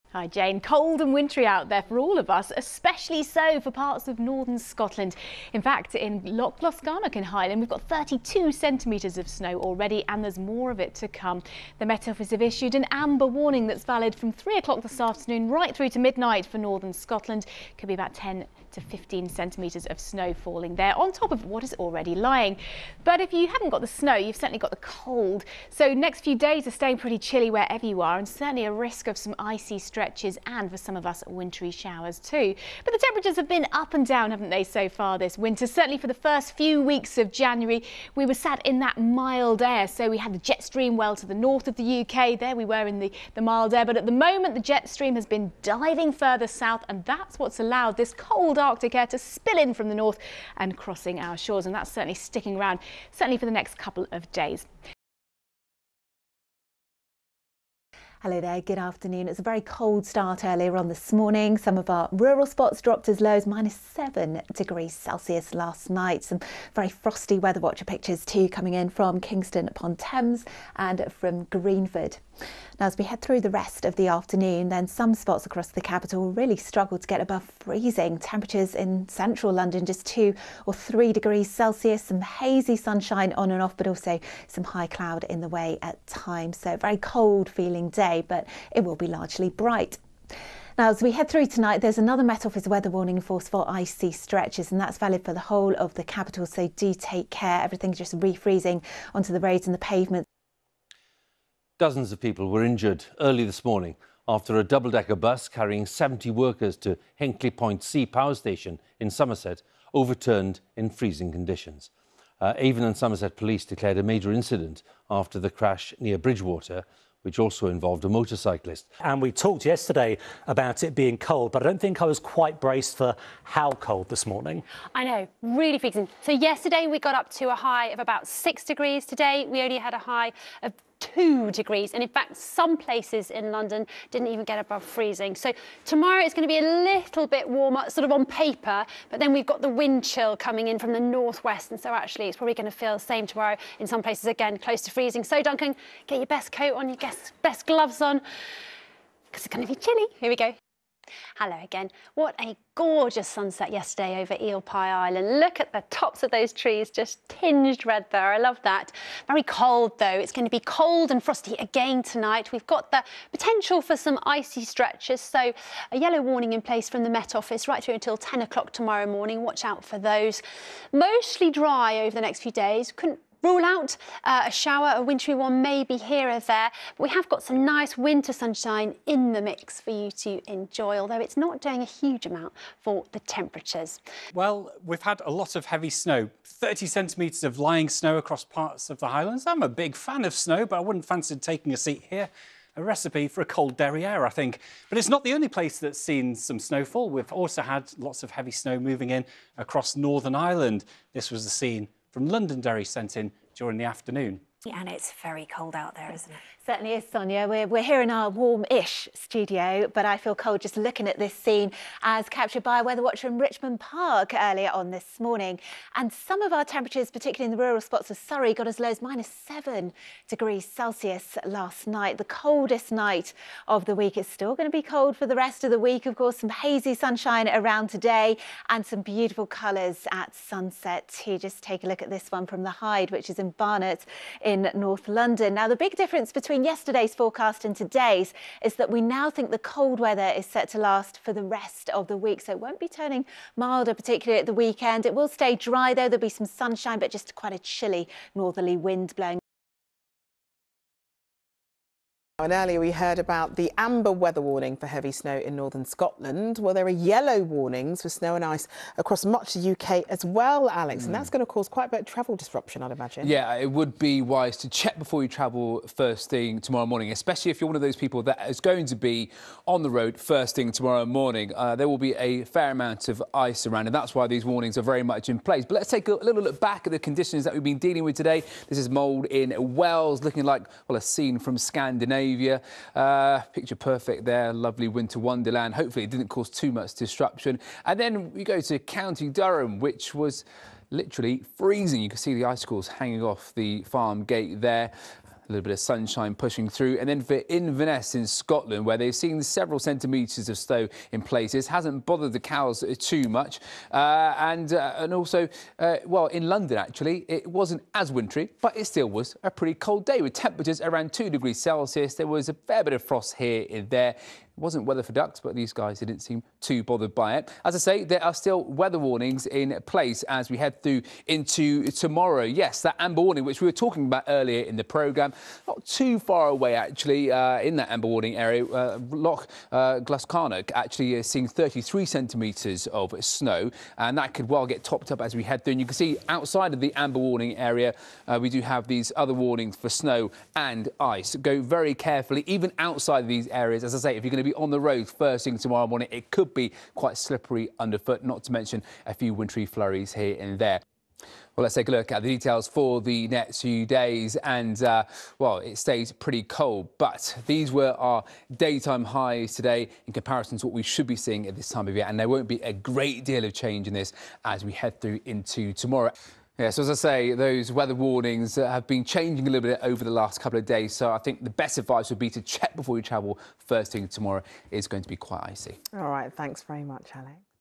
Weather images of the day - Cold, snow, icy and freezing (UK) - BBC&ITV - 17th January 2023
1) Listed and unlisted (library) weather images of the day (UK). (Unlisted)
2) Any outlooks and charts, etc. (if any).
3) Freezing ice and accidents and some video images included.

By evening.